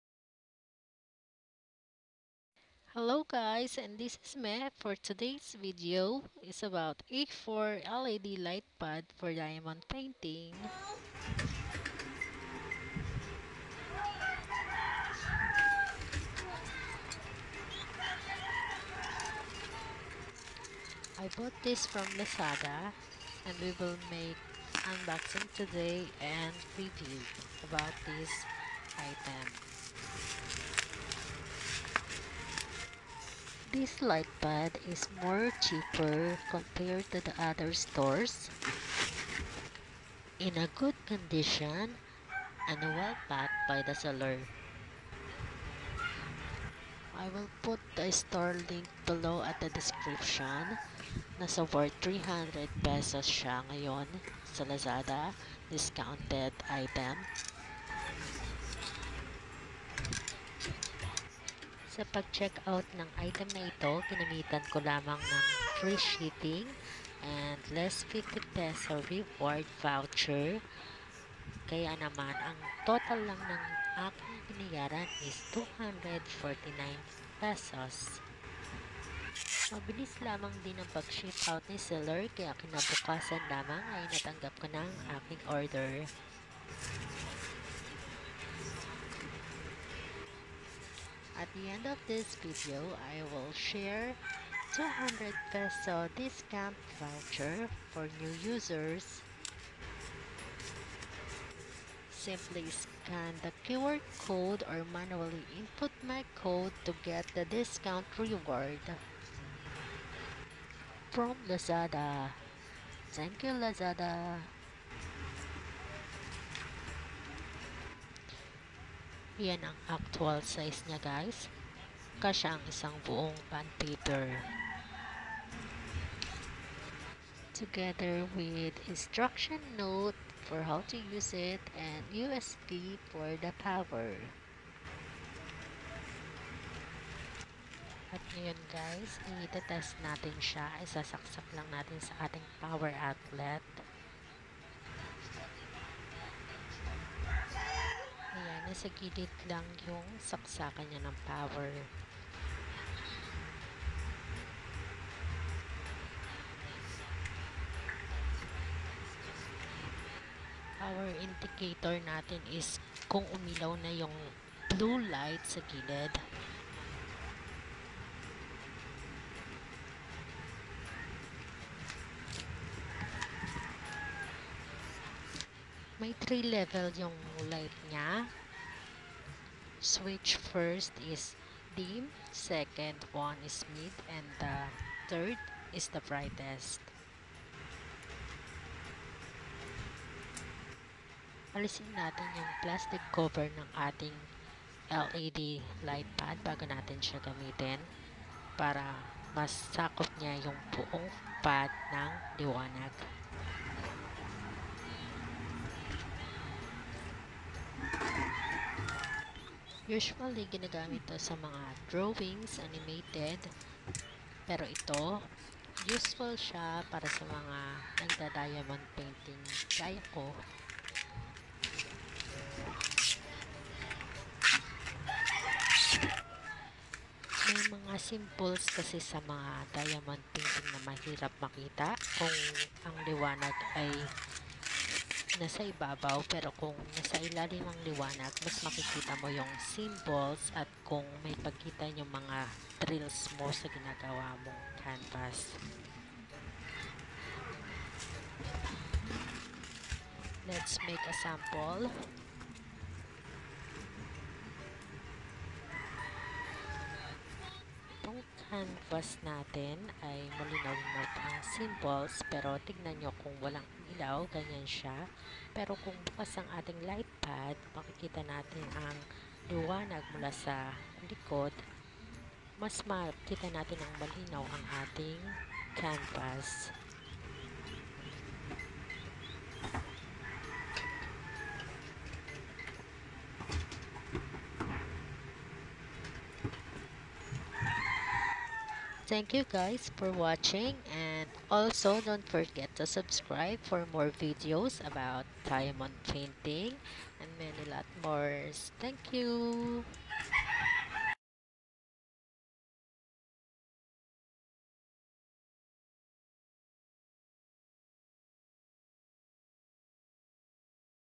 Cheaper A4 LED Light Pad for Diamond Painting (Redeem Lazada Discount Voucher)
This A4 LED LightPad is more CHEAPER compare to the other Stores.
And the Shipment is so fast Well packed and in Goog Condition..!

I bought for only ₱249

- -or- -

In LAZADA App
Go to Account and
EnTer and ReDeem Code
000323OD